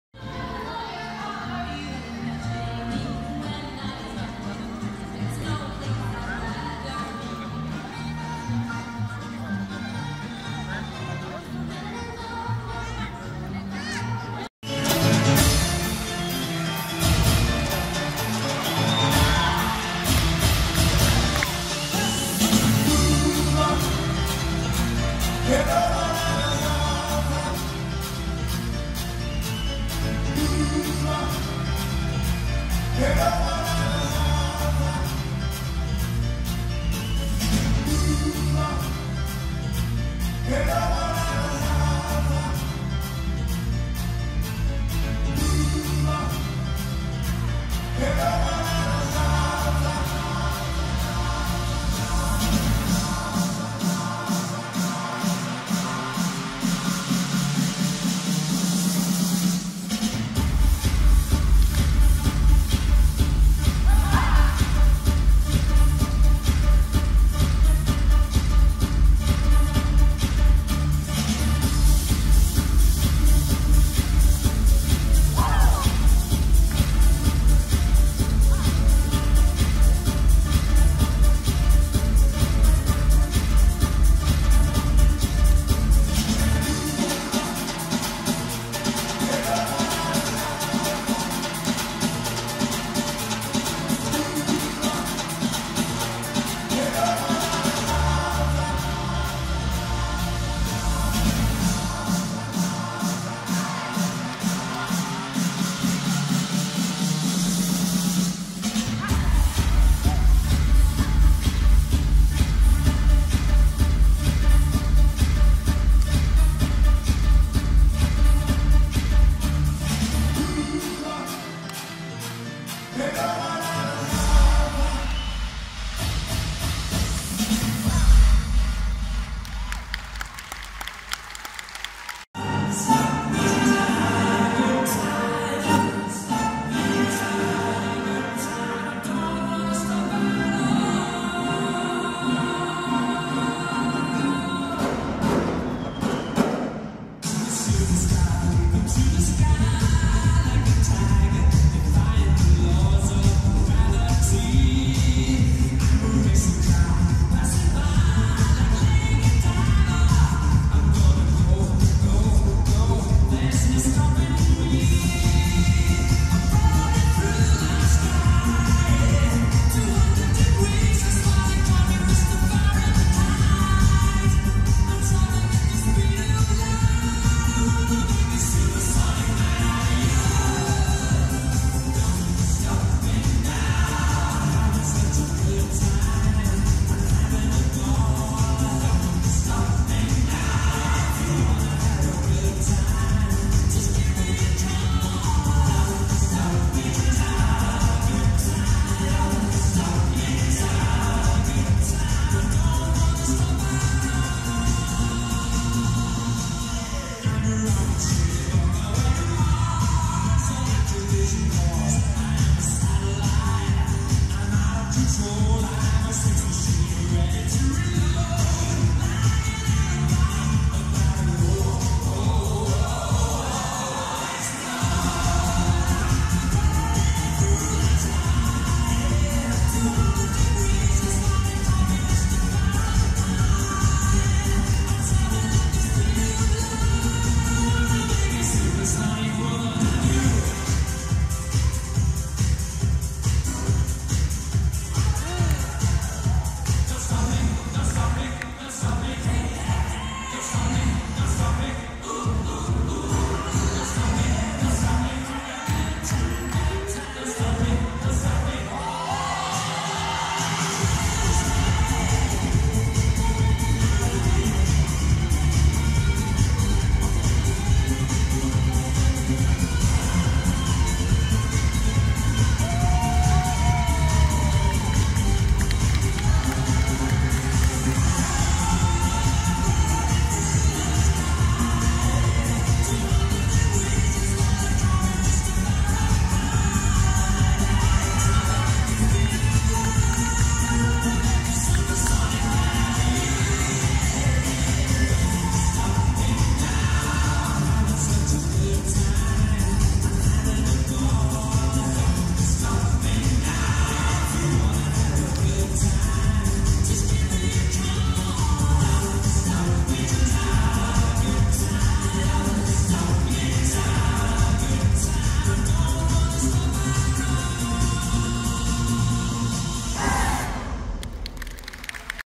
Naga World Show: High quality in Phnom Penh
Naga world casino complex have managed to produce a surprisingly high quality evening entertainment show. This is by far the highest quality production I've ever seen in Phnom Penh in an epic venue that wouldn't look out of place in London's west end. I attended on the opening night as a guest of one of the stars of the show.